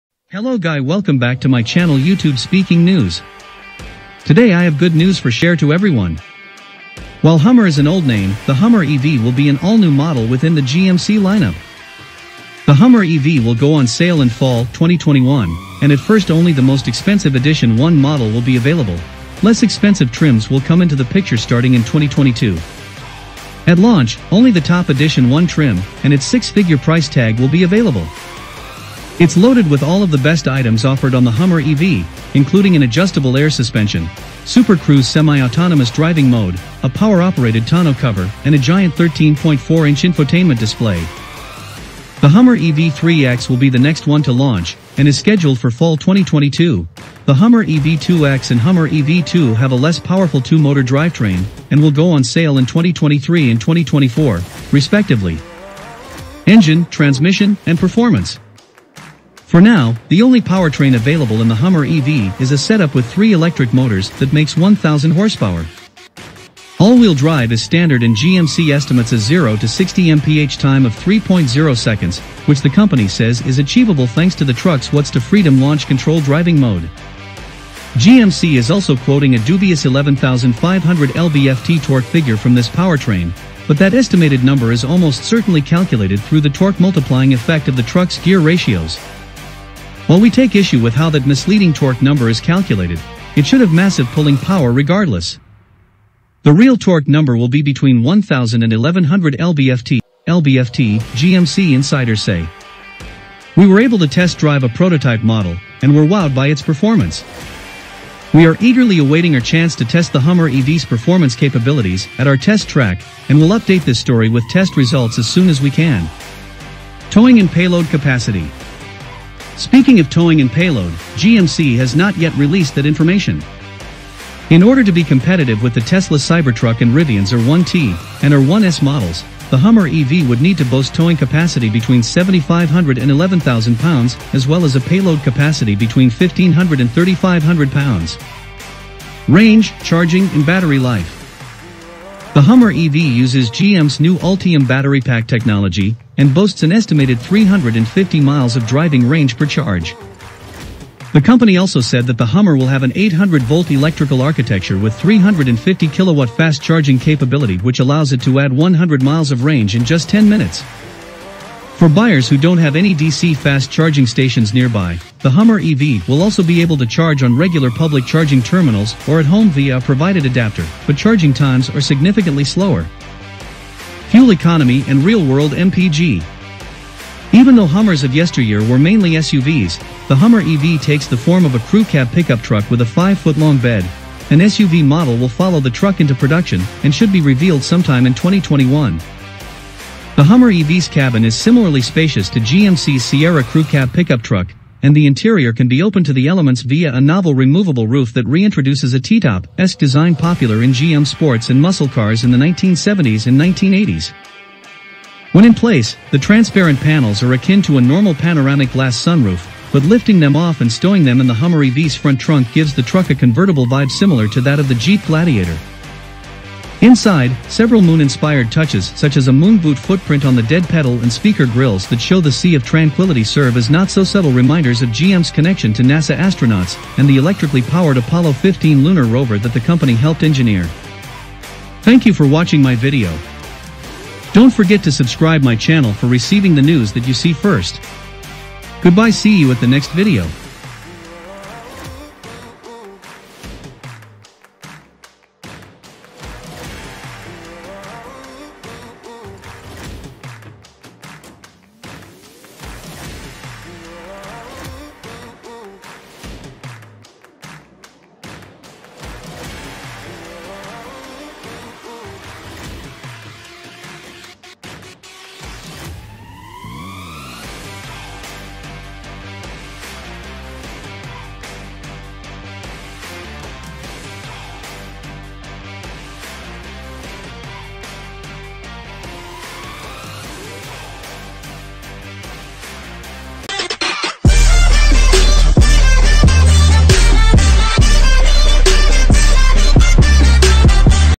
2022 GMC Hummer EV | Review Interior and Exterior | Engine | Transmission | and Performance.
Today I have good news for share to everyone.

While Hummer is an old name, the Hummer EV will be an all-new model within the GMC lineup. The Hummer EV will go on sale in fall, 2021, and at first only the most expensive Edition 1 model will be available; less expensive trims will come into the picture starting in 2022.
At launch, only the top Edition 1 trim and its six-figure price tag will be available. It's loaded with all of the best items offered on the Hummer EV, including an adjustable air suspension, Super Cruise semi-autonomous driving mode, a power-operated tonneau cover, and a giant 13.4-inch infotainment display. The Hummer EV3X will be the next one to launch and is scheduled for fall 2022; the Hummer EV2X and Hummer EV2 have a less powerful two-motor drivetrain and will go on sale in 2023 and 2024, respectively.
For now, the only powertrain available in the Hummer EV is a setup with three electric motors that makes 1000 horsepower. All-wheel drive is standard and GMC estimates a zero-to-60-mph time of 3.0 seconds, which the company says is achievable thanks to the truck's Watts to Freedom launch control driving mode. GMC is also quoting a dubious 11,500 lb-ft torque figure from this powertrain, but that estimated number is almost certainly calculated through the torque-multiplying effect of the truck's gear ratios. While we take issue with how that misleading torque number is calculated, it should have massive pulling power regardless. The real torque number will be between 1000 and 1100 lb-ft, GMC insiders say. We were able to test drive a prototype model and were wowed by its performance. We are eagerly awaiting our chance to test the Hummer EV's performance capabilities at our test track and will update this story with test results as soon as we can.
Towing and Payload Capacity.
Speaking of towing and payload, GMC has not yet released that information. In order to be competitive with the Tesla Cybertruck and Rivian's R1T and R1S models, the Hummer EV would need to boast towing capacity between 7500 and 11,000 pounds as well as a payload capacity between 1500 and 3500 pounds.

Range, Charging, and Battery Life.
The Hummer EV uses GM's new Ultium battery-pack technology and boasts an estimated 350 miles of driving range per charge. The company also said that the Hummer will have an 800-volt electrical architecture with 350-kilowatt fast-charging capability which allows it to add 100 miles of range in just 10 minutes. For buyers who don't have any DC fast charging stations nearby, the Hummer EV will also be able to charge on regular public charging terminals or at home via a provided adapter, but charging times are significantly slower.

Even though Hummers of yesteryear were mainly SUVs, the Hummer EV takes the form of a crew cab pickup truck with a five-foot-long bed; an SUV model will follow the truck into production and should be revealed sometime in 2021. The Hummer EV's cabin is similarly spacious to GMC's Sierra crew cab pickup truck, and the interior can be opened to the elements via a novel removable roof that reintroduces a T-top–esque design popular in GM sports and muscle cars in the 1970s and 1980s. When in place, the transparent panels are akin to a normal panoramic glass sunroof but lifting them off and stowing them in the Hummer EV's front trunk gives the truck a convertible vibe similar to that of the Jeep Gladiator. Inside, several moon-inspired touches—such as a moon-boot footprint on the dead pedal and speaker grilles that show the Sea of Tranquility—serve as not-so-subtle reminders of GM's connection to NASA astronauts and the electrically powered Apollo 15 lunar rover that the company helped engineer.